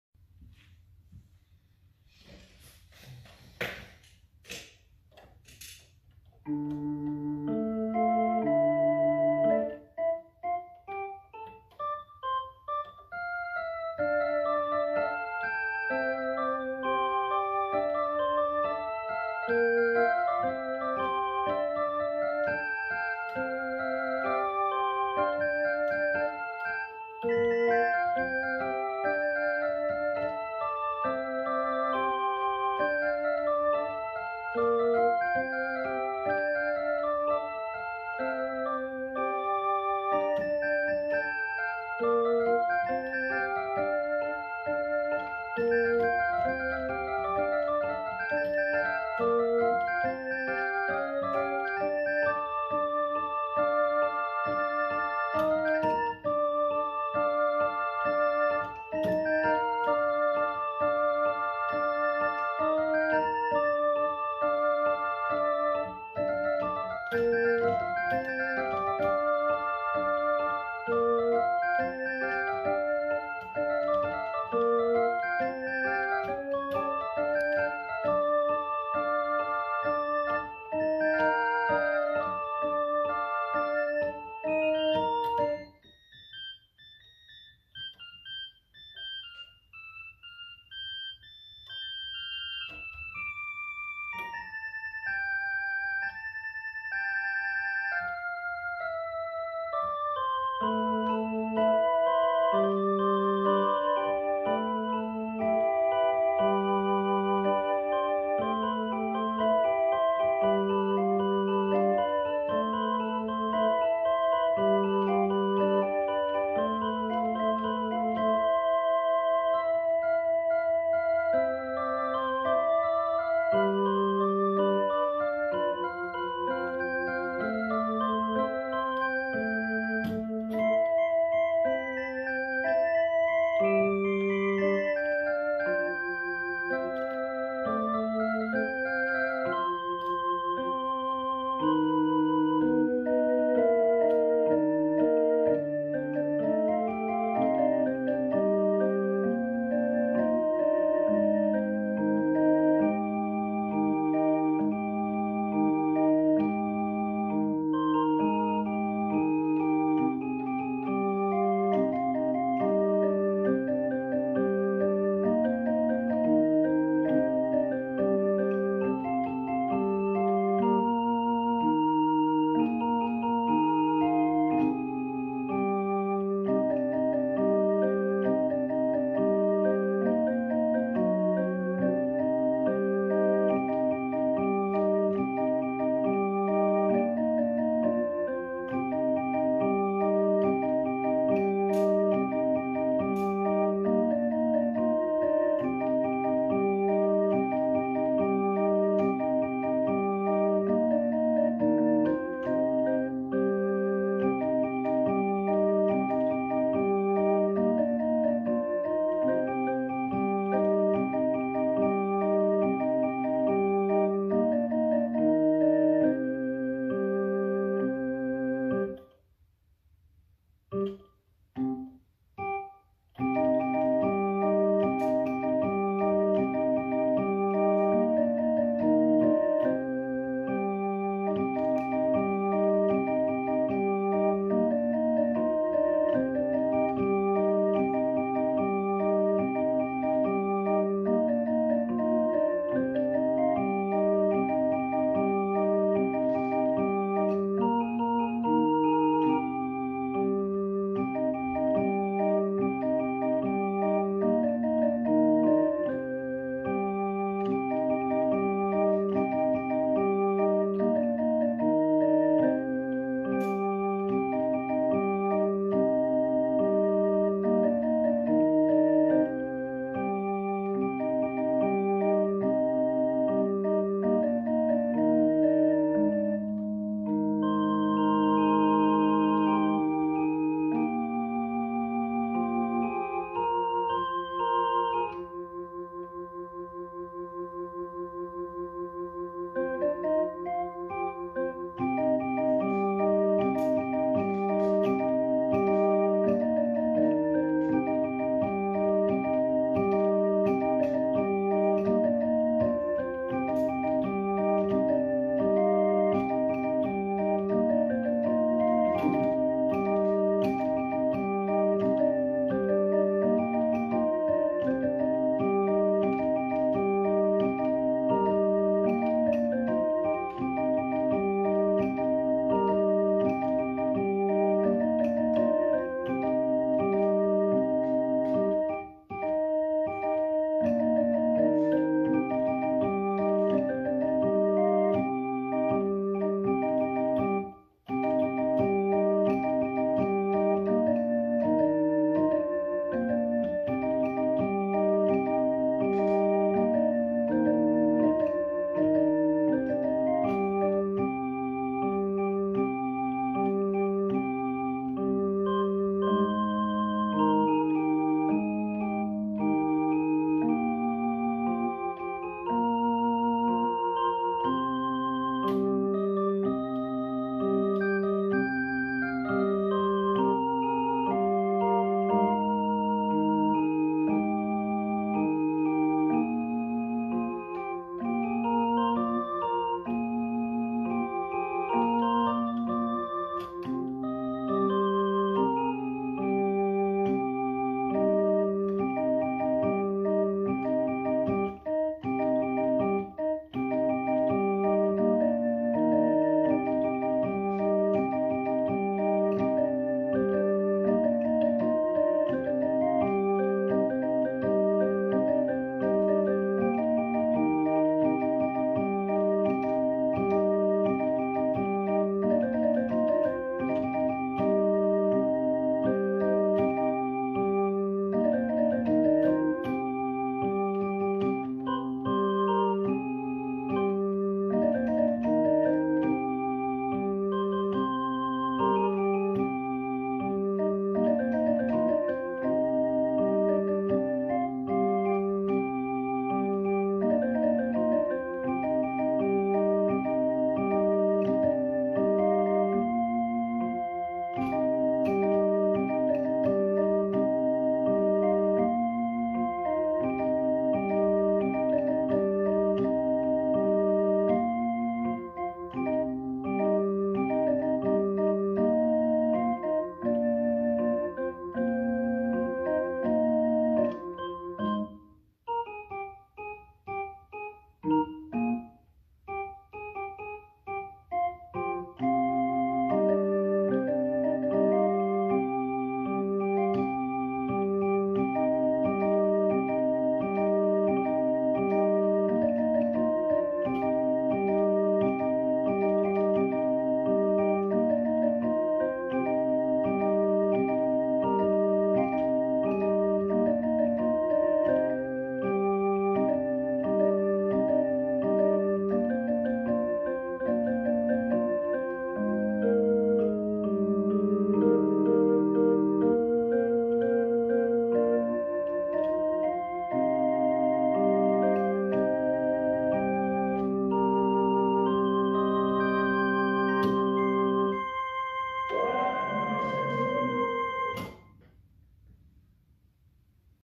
04 E. Organ
Here's an example of taking a little while to get in a groove.   Found a sound I liked around 3 minutes in and just stayed with it